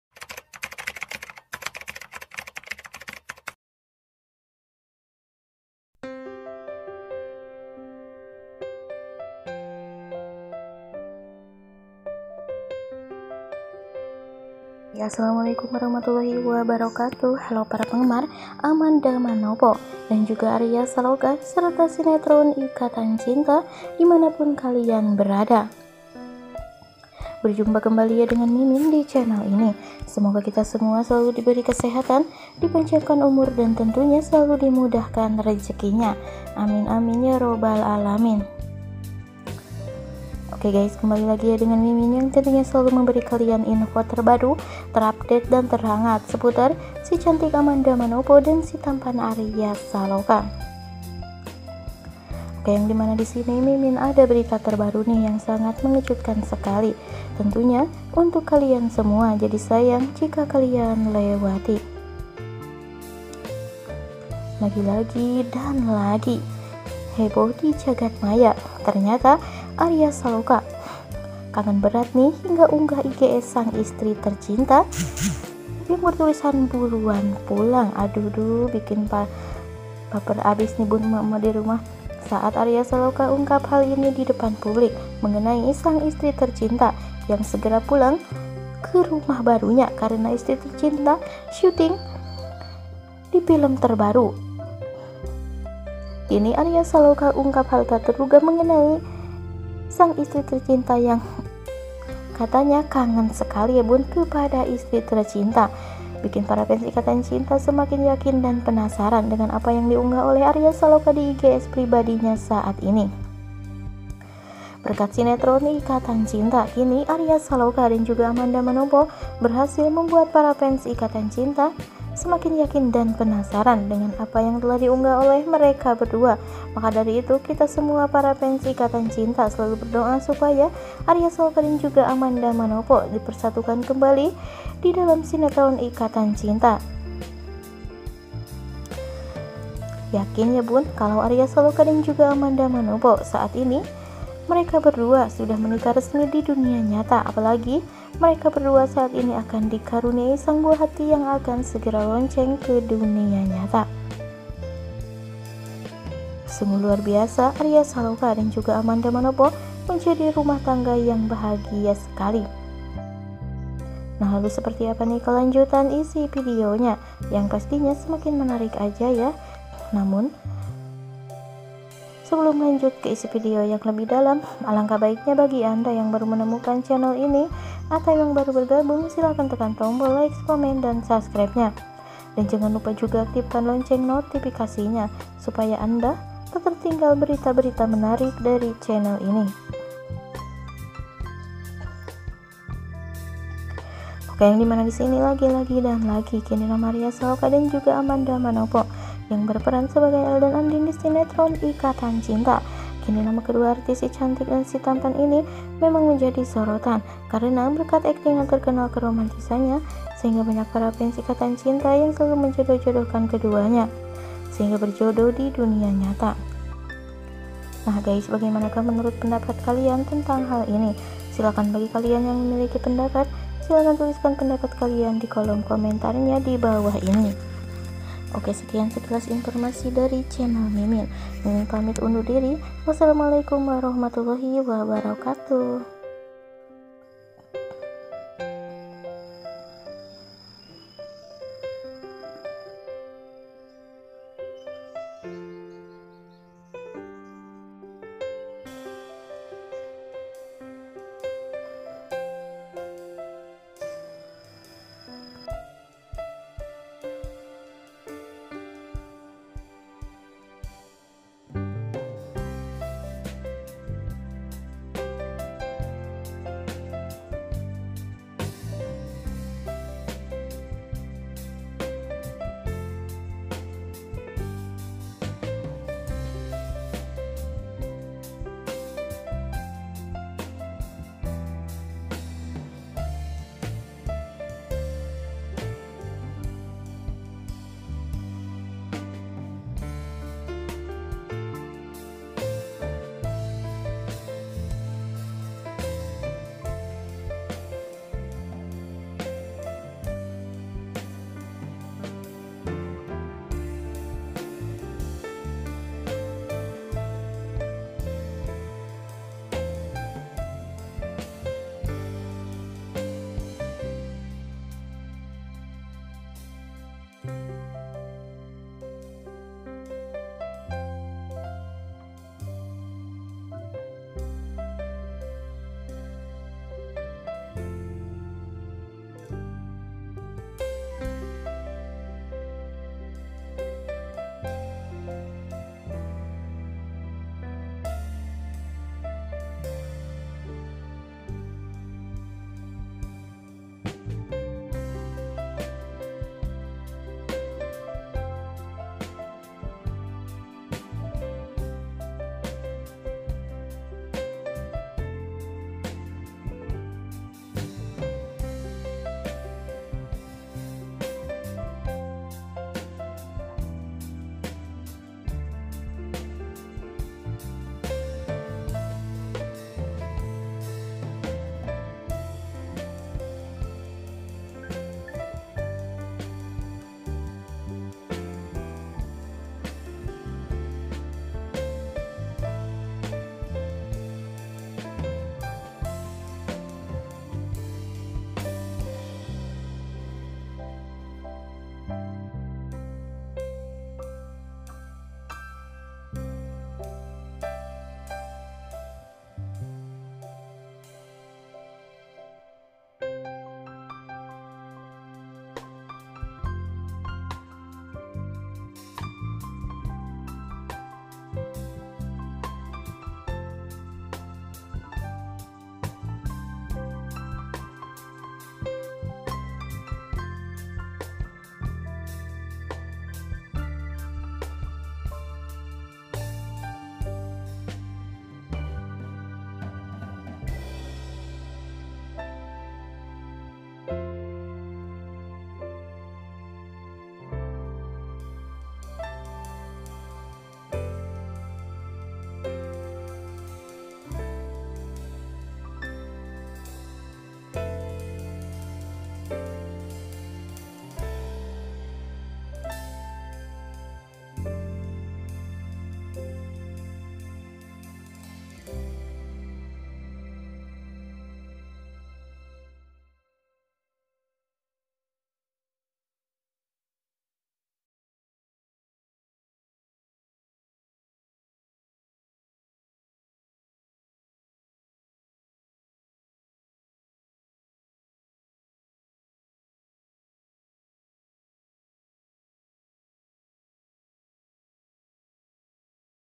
Heboh IGS Arya saloka bikin Ema Ema baper yang kata nya rindu berat sang istri tercinta ternyata..
__SEKIAN & TERIMAKASIH___